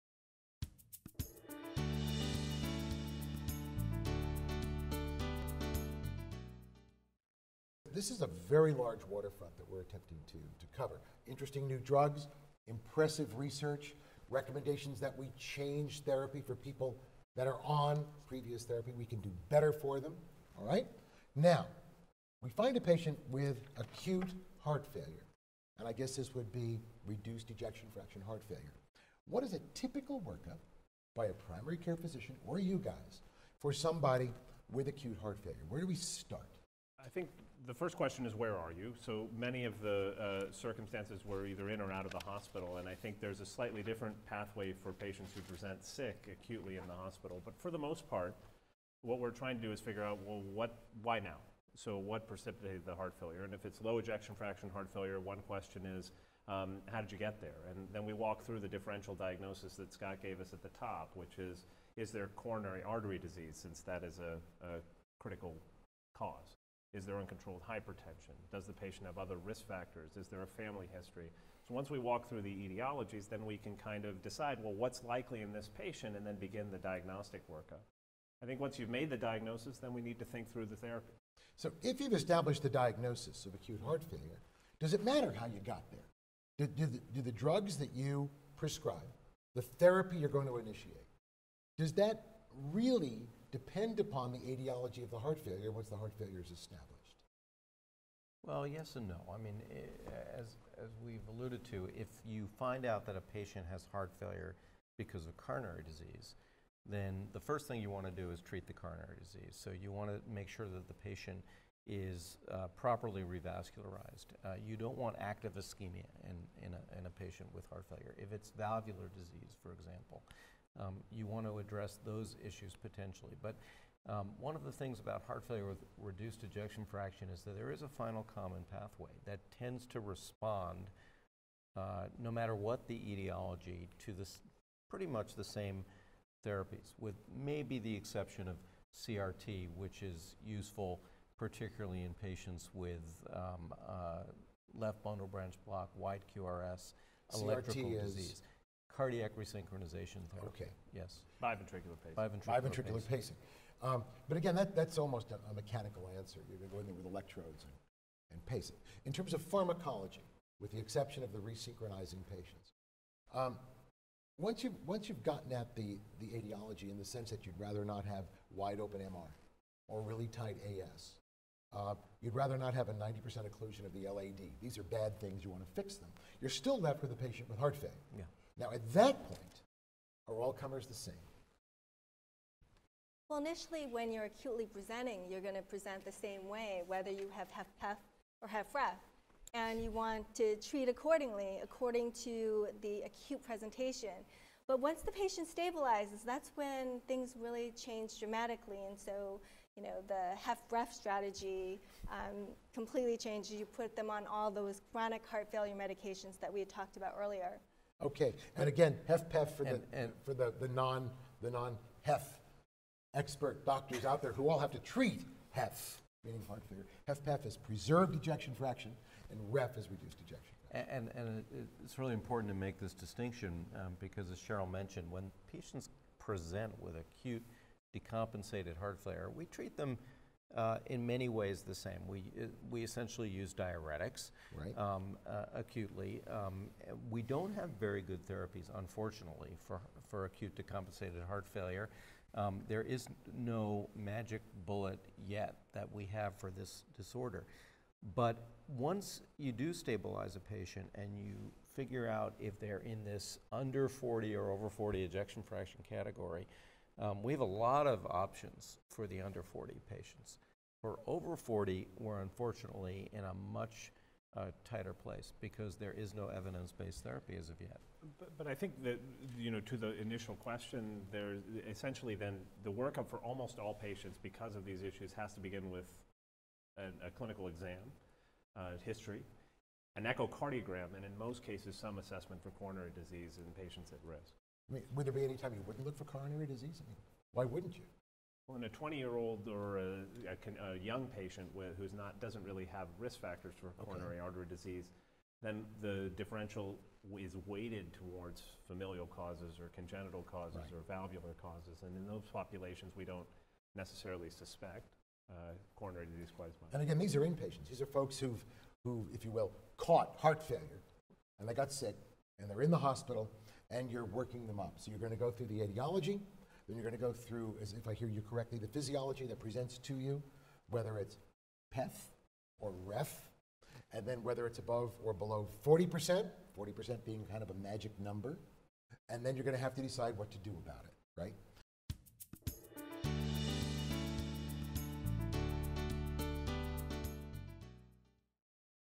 Acute Decompensated Heart Failure Hospitalization
In this segment, Sheryl Chow, PharmD, BCPS; Akshay Desai, MD; Peter L. Salgo, MD; and Scott Solomon, MD, review the typical workup for patients who are admitted to the hospital with acute decompensated heart failure.